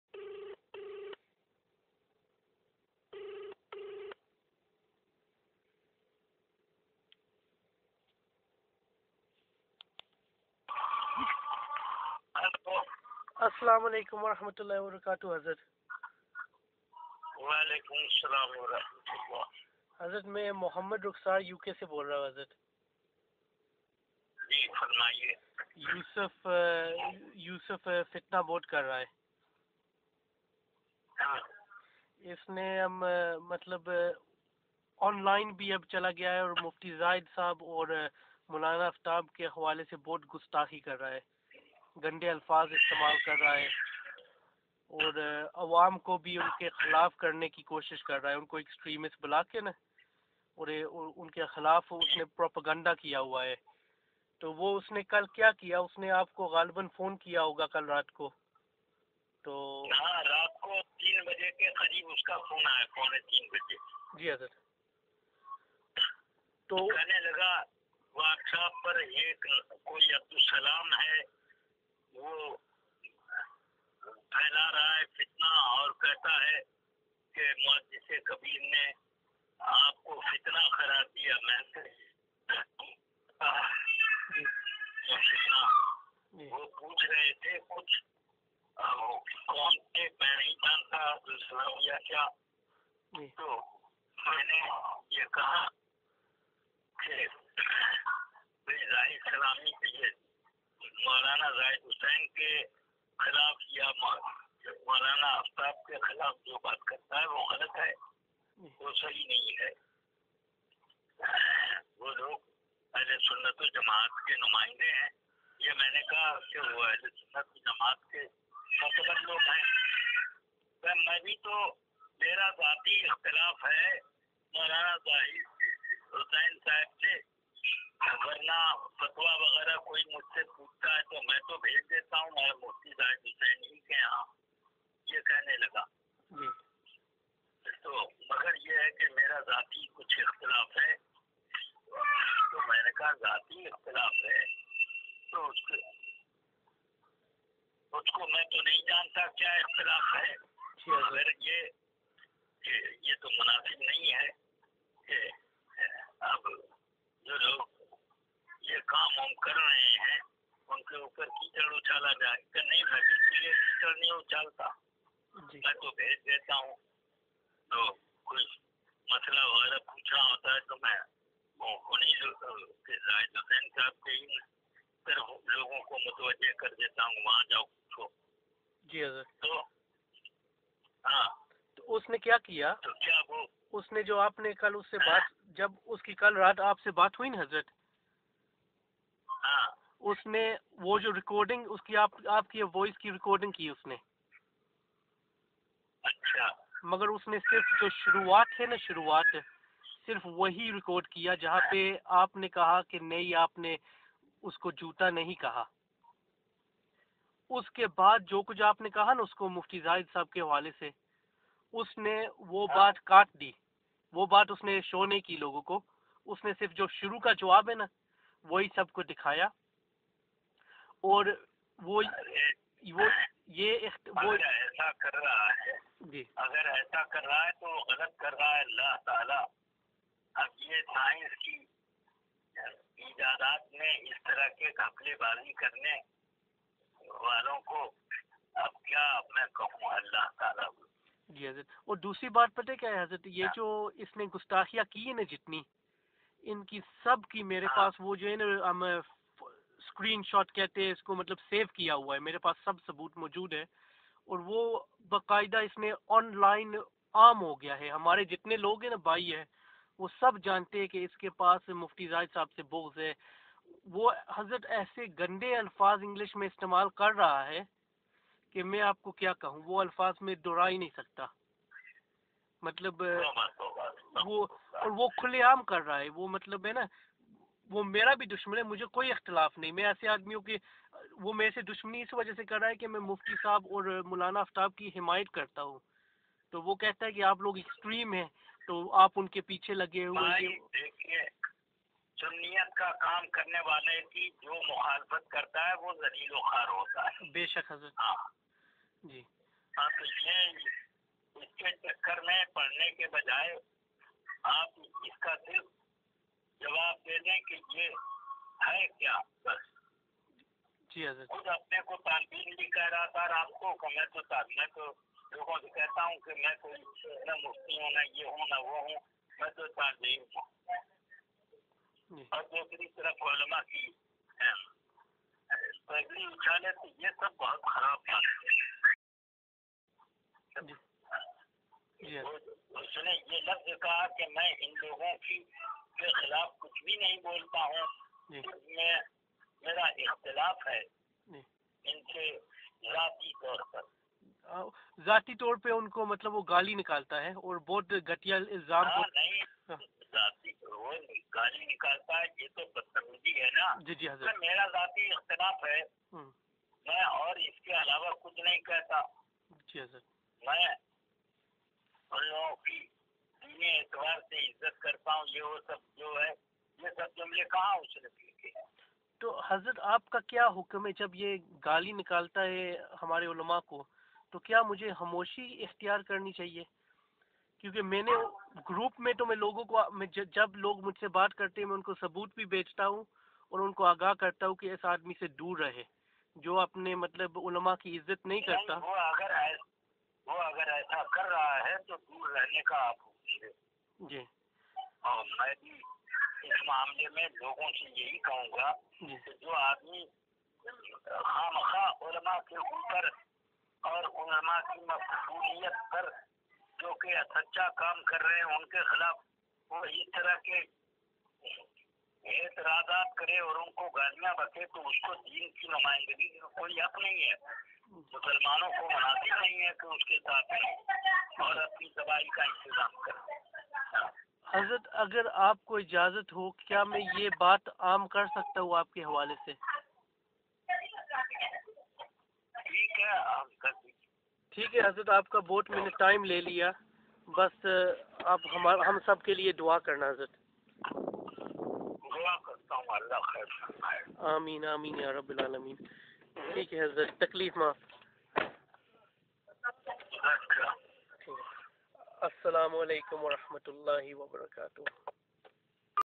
The truth about Yusuf Murray ― Conversation with al-Muhaddith al-Kabir
We spoke to al-Muhaddith al-Kabir ‘Allamah Ziya’ al-Mustafa al-Qadiri regarding Yusuf Murray and his abuse. We asked whether people who attack ‘Ulama like Mawlana Afthab Cassim and Mufti Zahid Hussain should be boycotted and they said yes, if they attack ‘Ulama like these then they should be boycotted and such people are not our representatives. After speaking to ‘Allamah Ziya’ al-Mustafa it was also clear that Yusuf Murray cut the clip where ‘Allamah Ziya’ al-Mustafa said to respect Mawlana Afthab Cassim and Mufti Zahid Hussain out and he told them lies that he was not attacking these ‘Ulama. He told ‘Allamah Ziya’ al-Mustafa he respects both and sends people to Mufti Zahid Hussain, when we have evidence he is telling people to avoid them. He cut the clip and hid ‘Allamah Ziya’ al-Mustafa’s advice.

Yusuf Murray acknowledged to ‘Allamah Ziya’ al-Mustafa that his issue with Mufti Zahid Hussain is personal, yet claims online it is religious.